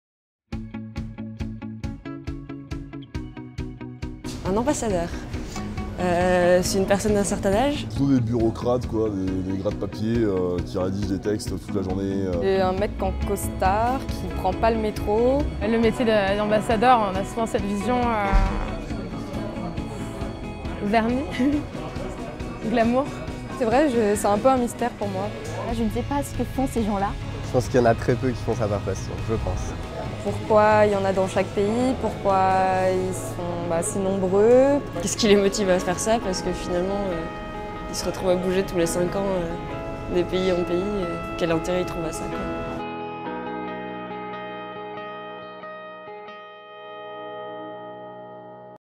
À la rencontre des ambassadeurs : "Pour vous, c'est quoi un ambassadeur ?"
Cette année, le ministère vous invite à participer à la rentrée diplomatique, en ouverture de la Conférence des ambassadeurs 2014. Pour la première fois, vous pourrez rencontrer et échanger avec des ambassadeurs en exercice : une occasion inédite de découvrir les coulisses de la diplomatie.

Instragram : @francediplo